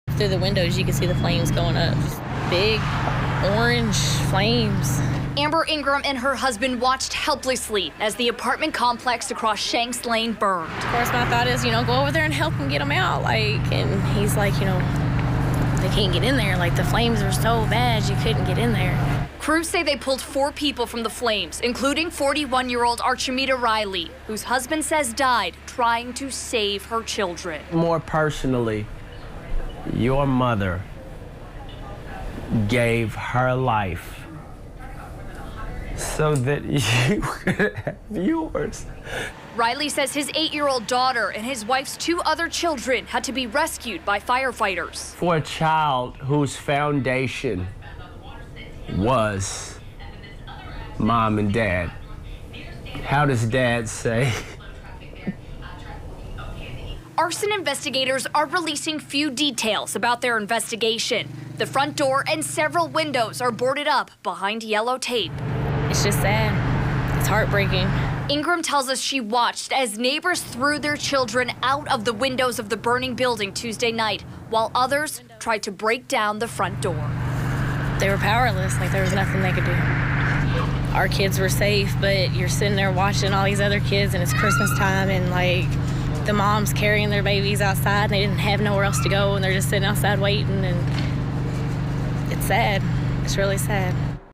Criminal investigation after 1 killed, 3 injured in fire
A criminal investigation is underway after a woman was killed in a fire and three others were sent to the hospital.